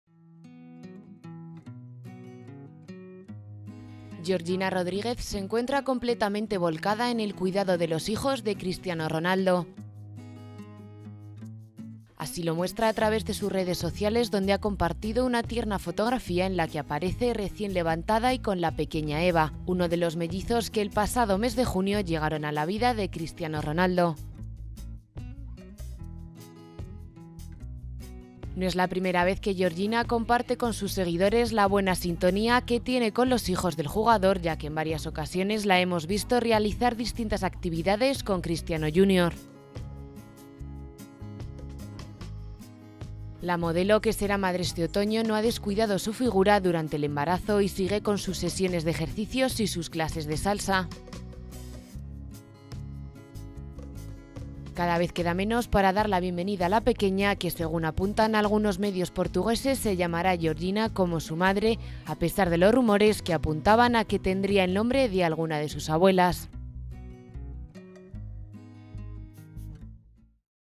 Georgina Rodríguez y su imagen más maternal con Eva
Georgina Rodríguez se encuentra completamente volcada en el cuidado de los hijos de Cristiano Ronaldo. Así lo muestra a través de sus redes sociales donde ha compartido una tierna fotografía en la que aparece recién levantada y con la pequeña Eva, una de los mellizos que el pasado mes de junio llegaron a la vida de Cristiano.